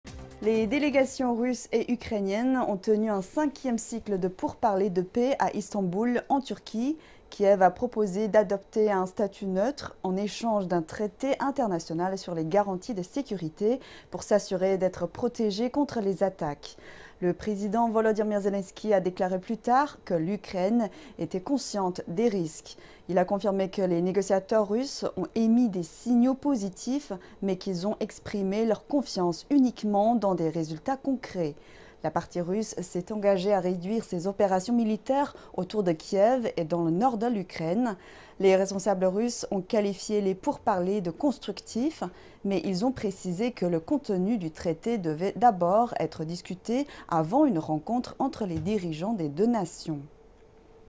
Rencontre Russie - Ukraine : négociations à Istanbul, de timides avancées
Les délégations russe et ukrainienne ont tenu un 5e cycle de pourparlers de paix à Istanbul, en Turquie. Kiev a proposé d'adopter un statut neutre, en échange d'un traité international sur les garanties de sécurité pour s'assurer d'être protégé contre les attaques. Le président Volodymyr Zelensky a déclaré plus tard que l'Ukraine était consciente des risques. Il a confirmé que les négociateurs russes ont émis des signaux positifs, mais qu'ils ont exprimé leur confiance uniquement dans des résultats concrets.